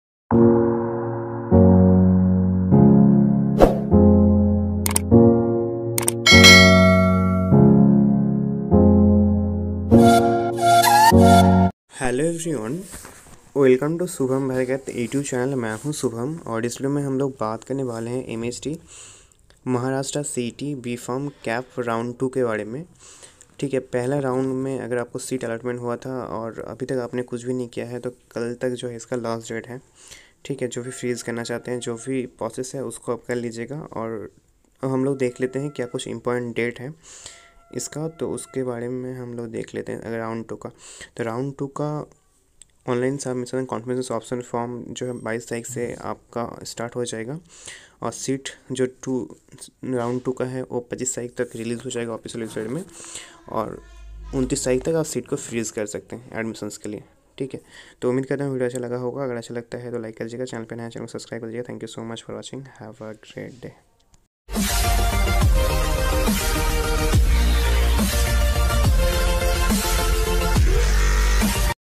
MHT CET 2020: Revised schedule for B.Pharm CAP Round 2 released online, Get Direct Link Here
The Maharashtra State Common Entrance Test Cell has released the CAP Round 2 revised schedule for admission to the MHT CET 2020 B.Pharm course.

According to the revised schedule released, the Revised Provisional Vacant Seats for CAP Round-II will be released on the official website on January 21, 2021. The last date for students to accept the seats offered to the candidates in the provisional admission list as per CAP Round 1 is January 20, 2021.

The revised MHT CET 2020 CAP Round 2 schedule is available on the official website - cetcell.mahacet.org. Candidates can also check the MHT CET B.Pharm Revised admission schedule through the direct link provided below.

MHT CET 2020 B.pharm Admissions - Official Notification

As per the schedule provided the last date for the Online Submission and Confirmation of Option Form of CAP Round-II is January 22, 2021, while the revised Provisional Allotment of CAP Round-II will be available on the official website on January 25, 2021. The last date for students to accept the offered seat according to the Allotment of CAP Round II is January 29, 2021.